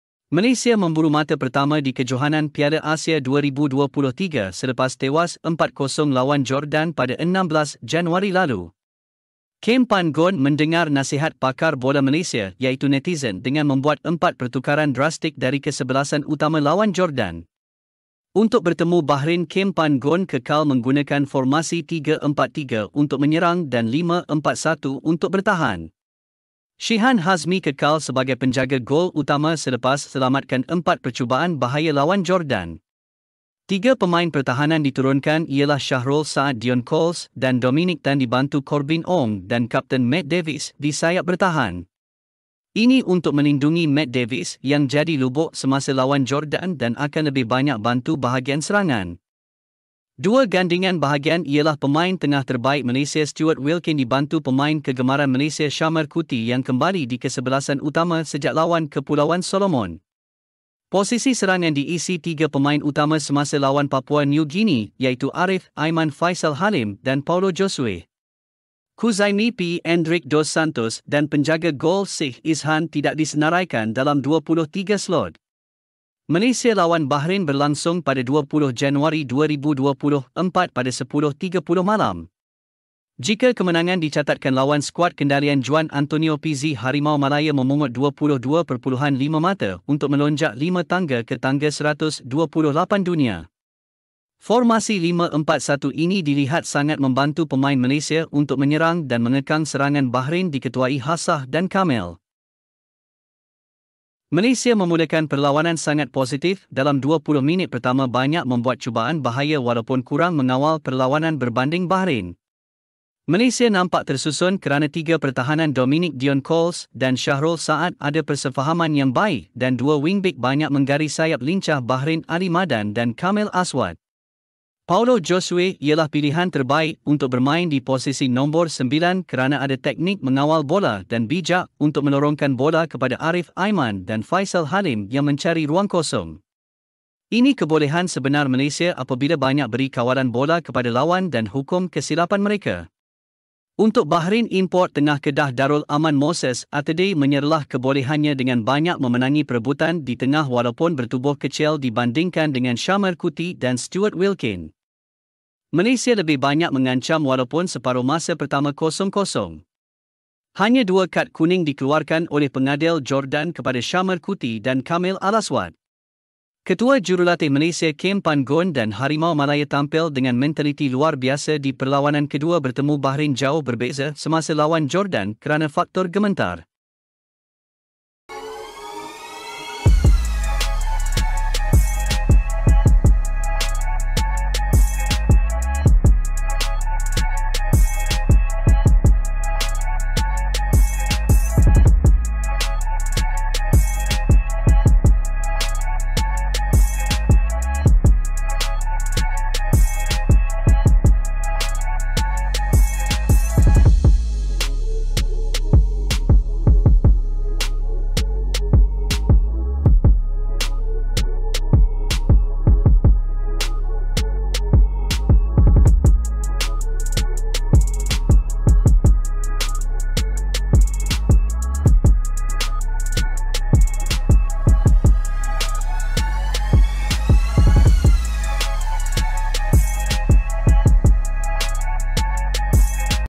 Malaysia JATUH ke 136 dunia | Bahrain 1 - 0 Malaysia | Piala Asia 2023
Siapa paksa Kim Pan-gon bawa Natxo Insa ke Piala Asia 2023.

Sorotan Perlawanan: Bahrain 1-0 Malaysia | Piala Asia 2023
AsianCup2023 | Group E : Bahrain 1-0 Malaysia
LIVE | AFC ASIAN CUP QATAR 2023™ | Bahrain 1-0 Malaysia